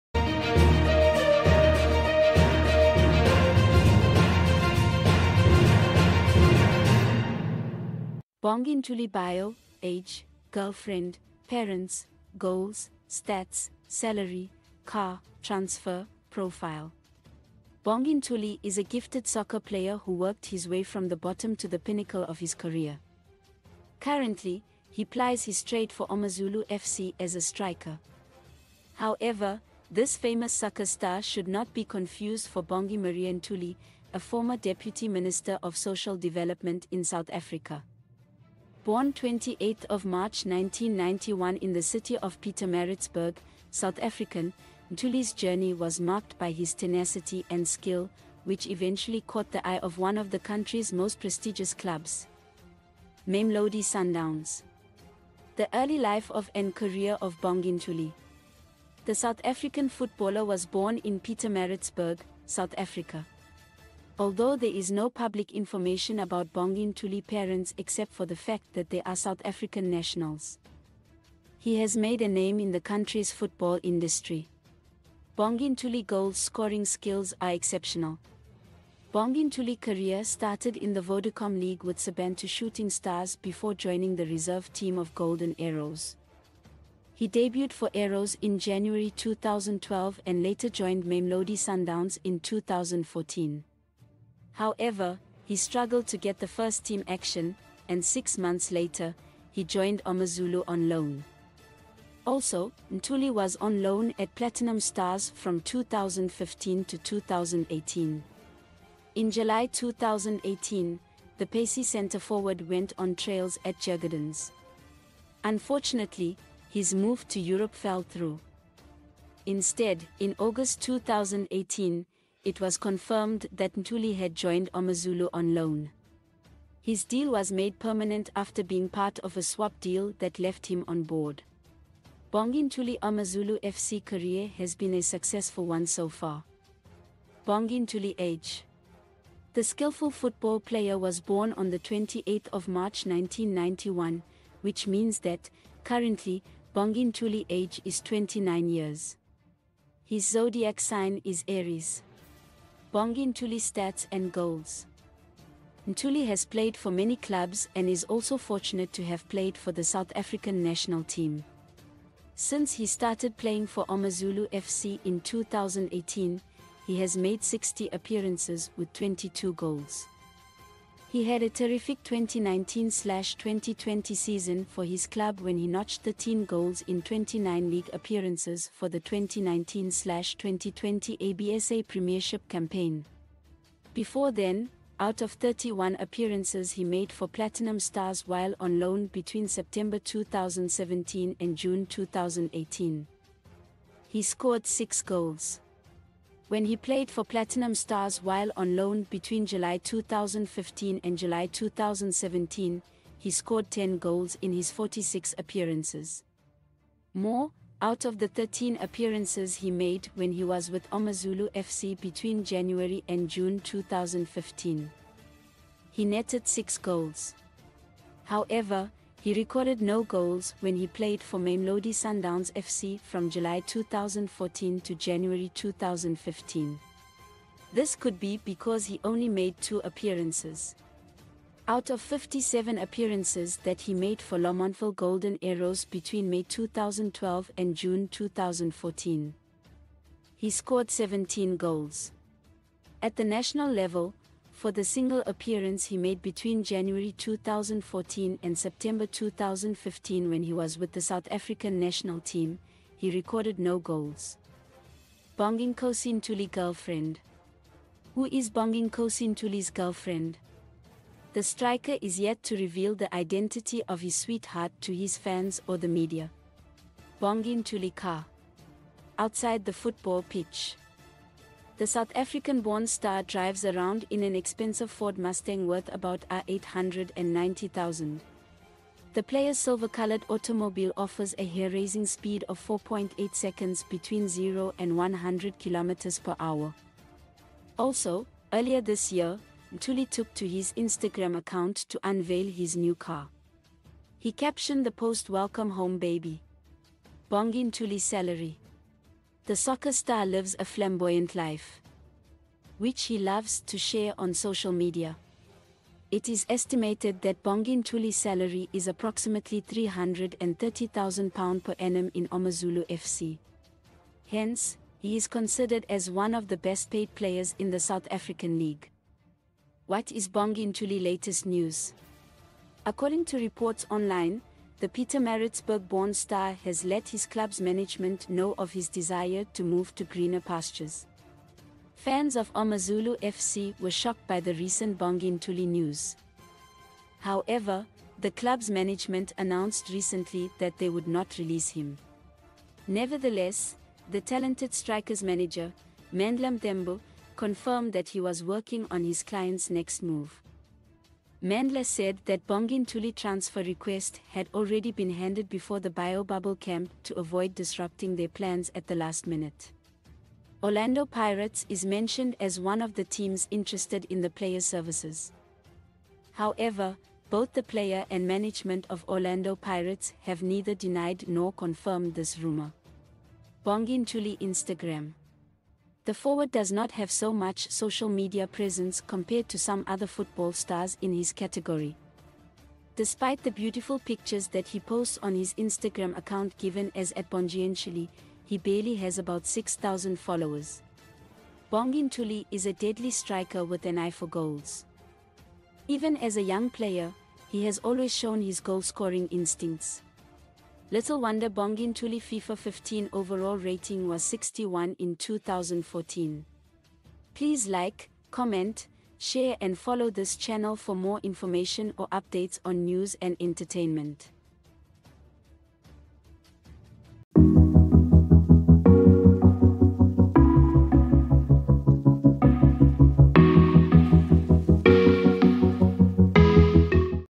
Bongi Ntuli Biograyphy: Age, Girlfriend, Parents, Goals, Stats, Salary, Car, Transfer, Profile
Bongi Ntuli is a gifted soccer player who worked his way from the bottom to the pinnacle of his career. Currently, he plies his trade for AmaZulu F.C. as a striker. However, this famous soccer star should not be confused for Bongi Maria Ntuli, a former Deputy Minister of Social Development in South Africa. Born 28th of March, 1991 in the city of Pietermaritzburg, South African, Ntuli’s journey was marked by his tenacity and skill, which eventually caught the eye of one of the country's most prestigious clubs, Mamelodi Sundowns.

The early life of and career of Bongi Ntuli:
The South African footballer was born in Pietermaritzburg, South Africa. Although there is no public information about Bongi Ntuli parents except for the fact that they are South African nationals, he has made a name in the country's football industry. Bongi Ntuli goals scoring skills are exceptional.

Bongi Ntuli career started in the Vodacom League with Sobantu Shooting Stars before joining the reserve team of Golden Arrows. He debuted for Arrows in January 2012 and later joined Mamelodi Sundowns in 2014. However, he struggled to get the first-team action, and six months later, he joined AmaZulu on loan. Also, Ntuli was on loan at Platinum Stars from 2015 to 2018.